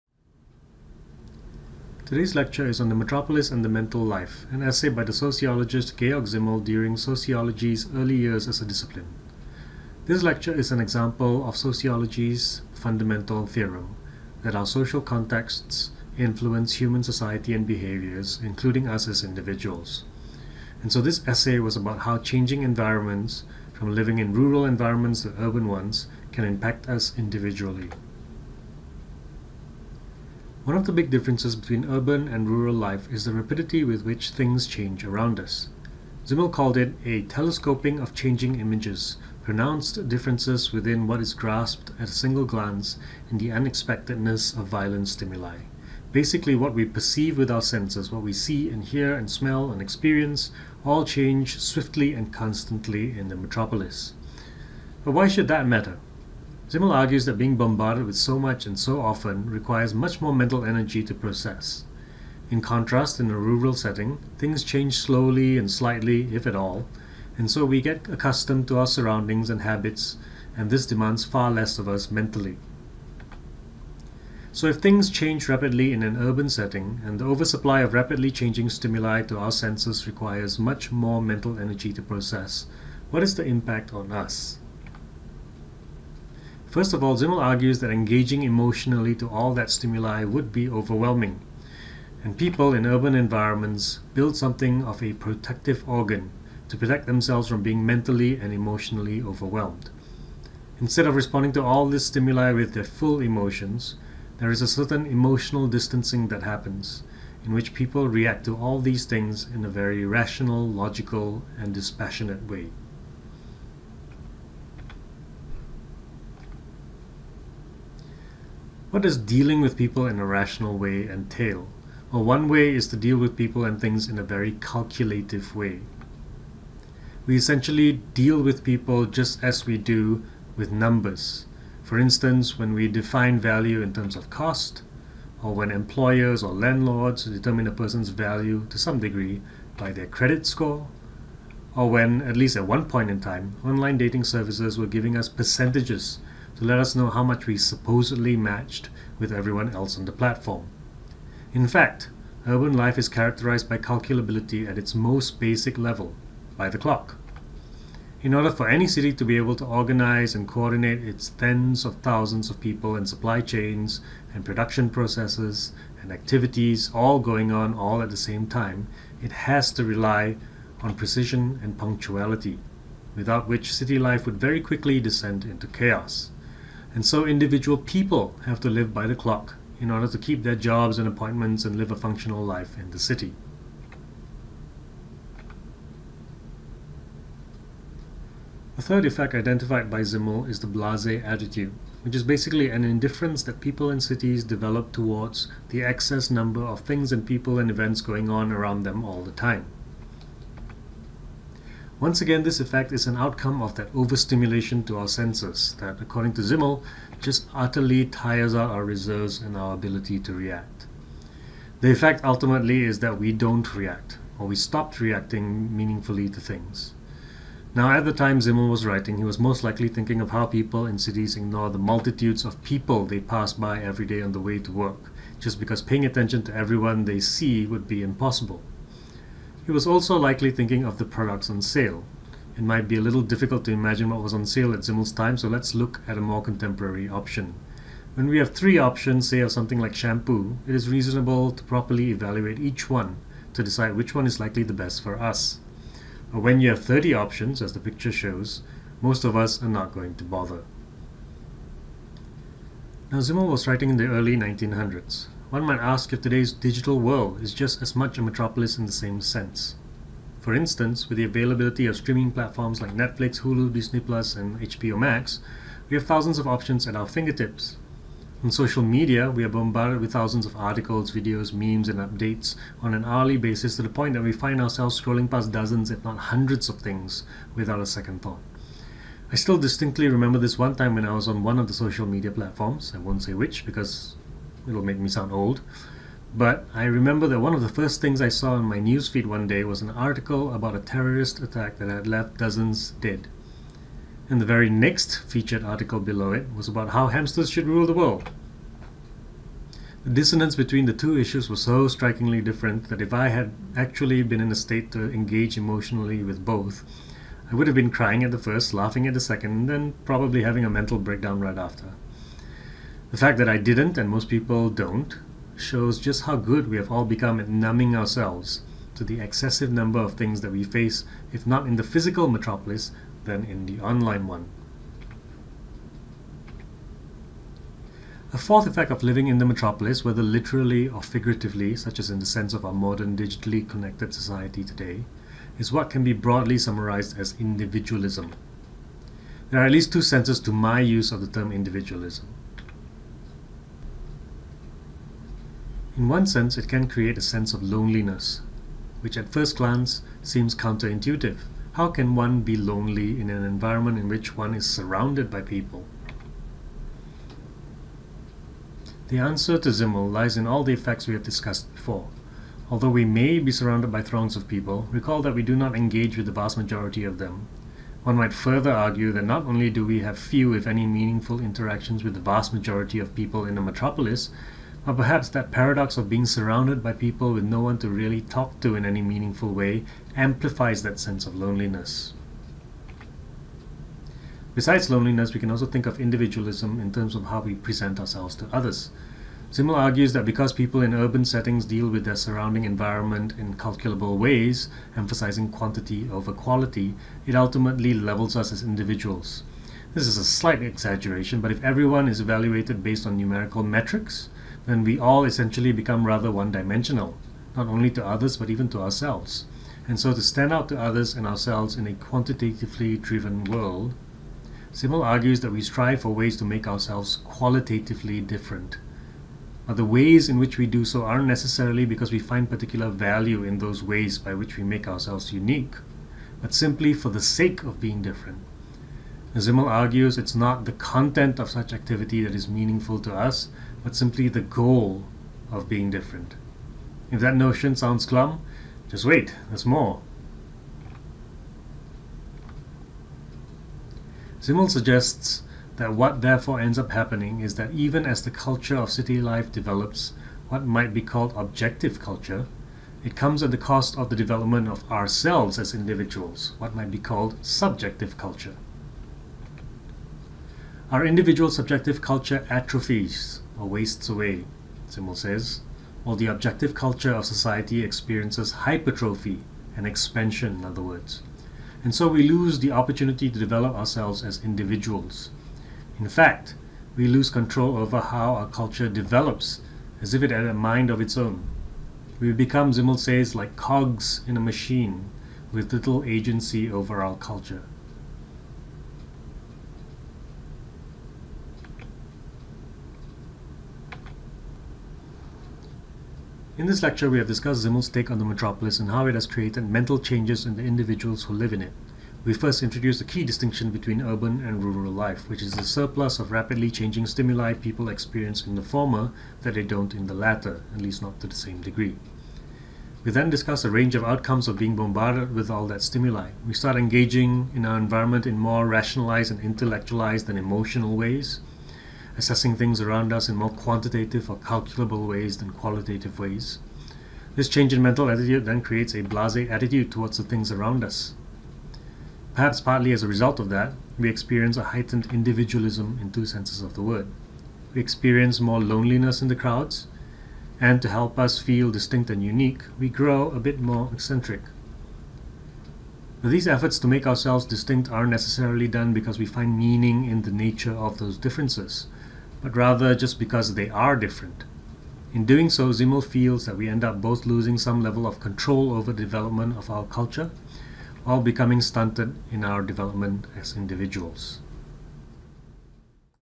SOC 285 - 5. The Metropolis and the Mental Life
Lecture on an excerpt from Georg Simmel's The Metropolis and the Mental Life.